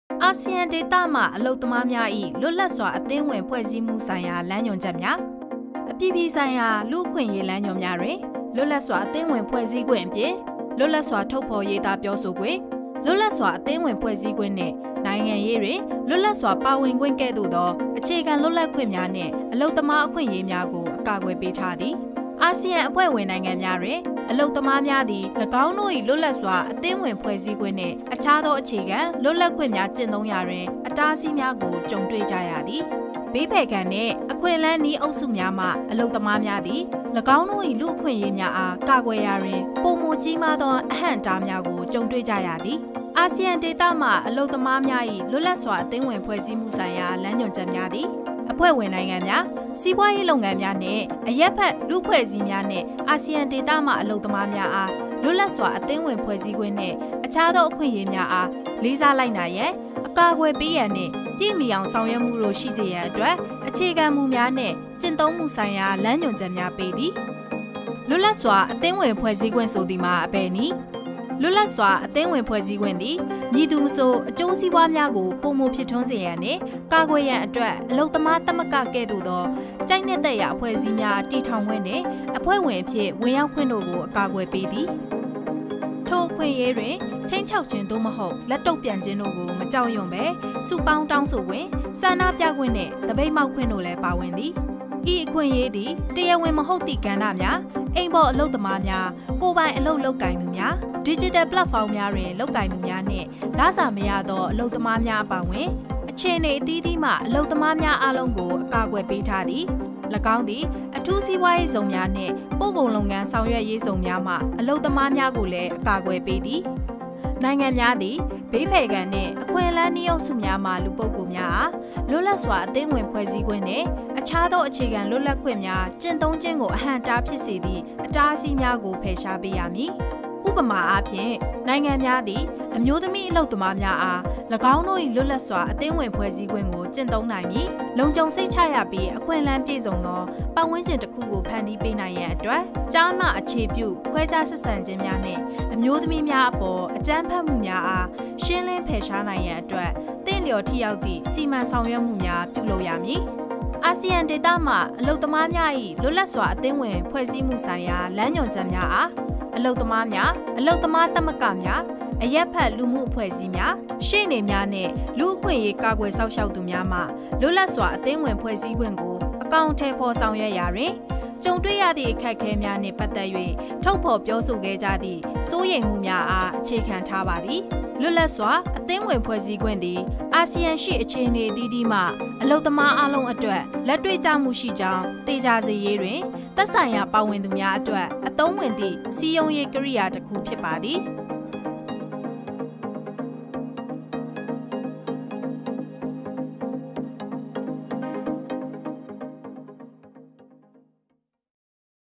Guidance on Freedom of Association of Workers in ASEAN - Myanmar
In order to improve respect for the rights of workers in Southeast Asia, the International Commission of Jurists (ICJ), together with the Business & Human Rights Resource Centre (BHRRC), will launch a new Guidance on Freedom of Association of Workers in ASEAN.